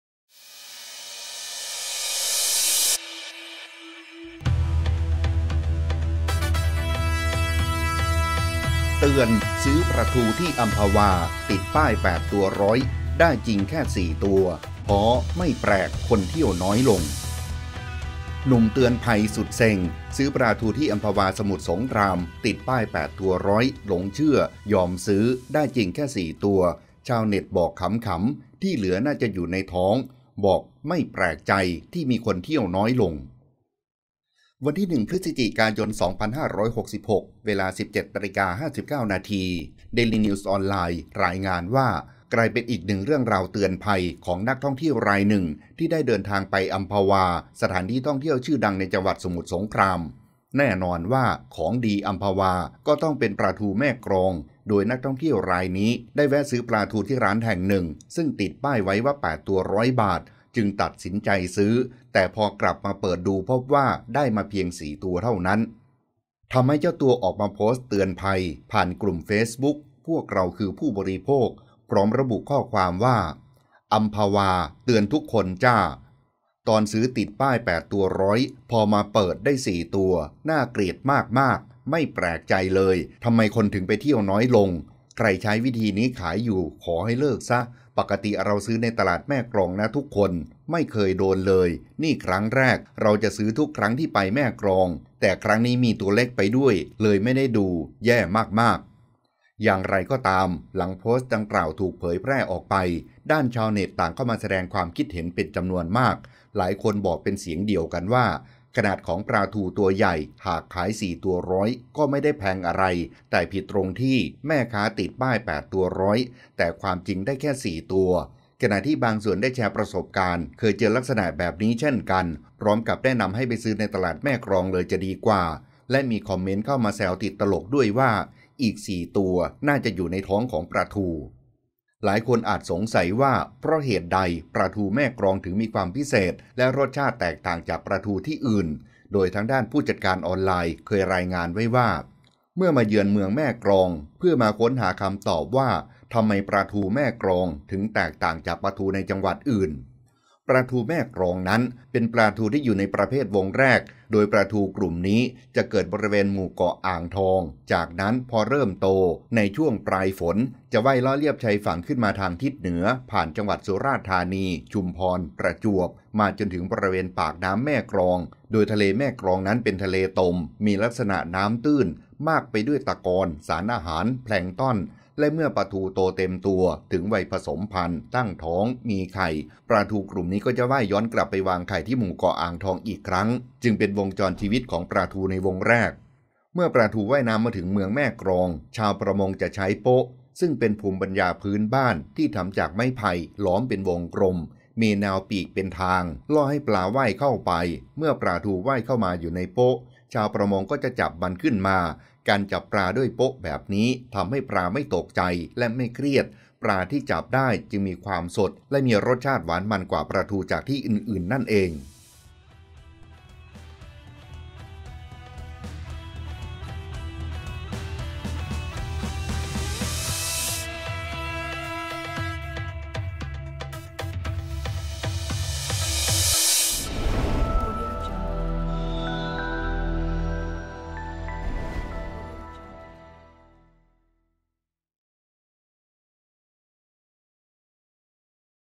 เตือน! ซื้อปลาทูที่อัมพวา ติดป้าย 8 ตัวร้อย ได้จริงแค่ 4 ตัว พ้อ! ไม่แปลกคนเที่ยวน้อยลง
ขอบคุณที่มา : dailynews , mgronline
ขอบคุณที่มาภาพ : FB-พวกเราคือผู้บริโภค